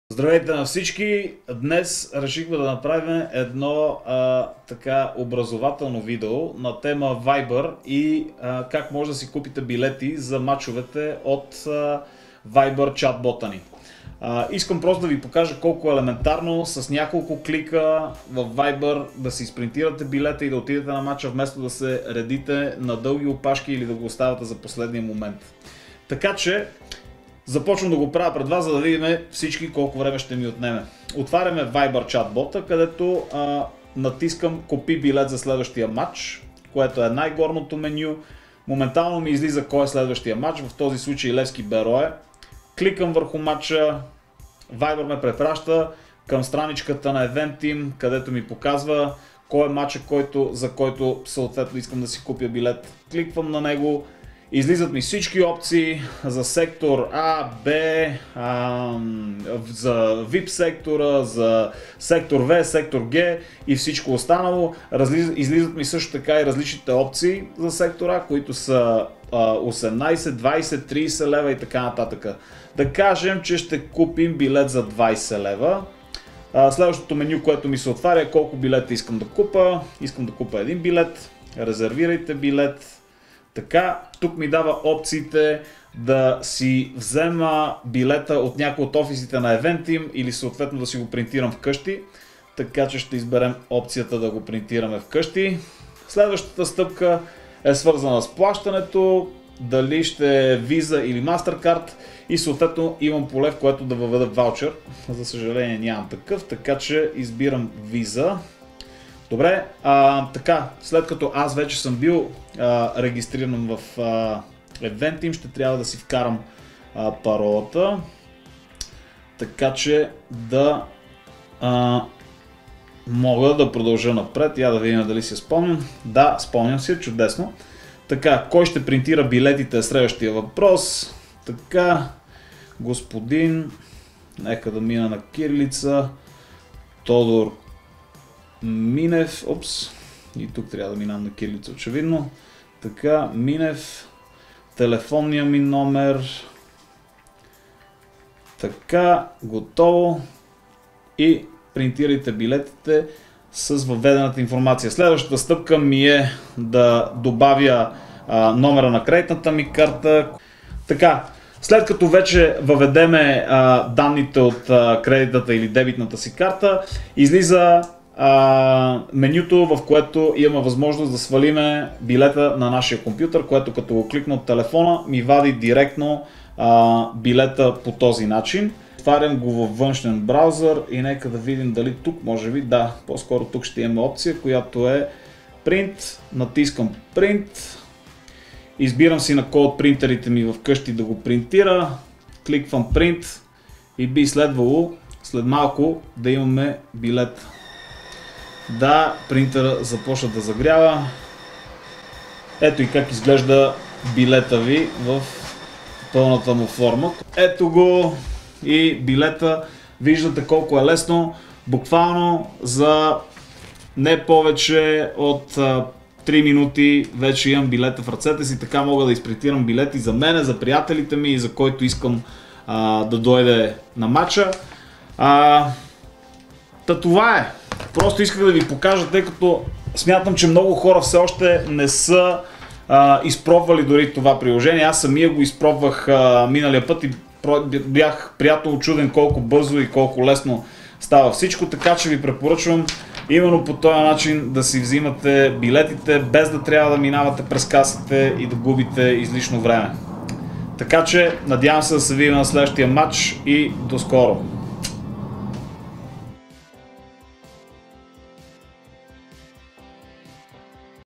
Как да си купим билет във Вайбър за броени минути?
Левскари, идва време за поредния мач у дома. Фланелката и шалът на любимия отбор стоят на закачалката и очакват деня и часа, в който отново ще стъпим на „Герена“. Там, където ще се срещнем с нашите приятели и ще се почувстваме отново част от „синьото“ семейство. И всичко това вече е още по-лесно осъществимо. Не е нужно да се редим по опашки и да закъсняваме за началото на двубоя, защото можем да си вземем пропуск за мач на „сините“ в няколко стъпки и буквално за броени минути!
Как става? Вижте в нашето видео!
САМО ЛЕВСКИ!